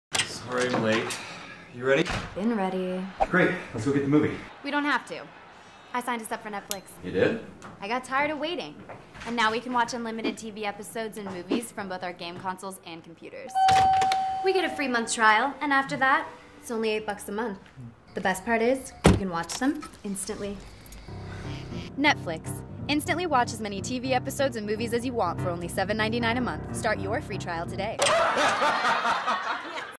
Netflix Commercial "Movie Night" By Chris Cordova
This was a commercial made for a Netflix contest throught Poptent.net
The commercial took place in February of 2011.
Directed and Edited By Chris Cordova
The Cinematographer was Jenn Gittings.
Special Thanks to:
Anthony Canello @TheAboy
Michael Vickers
Michael Griego
Derrick Bean
Sally Digirolama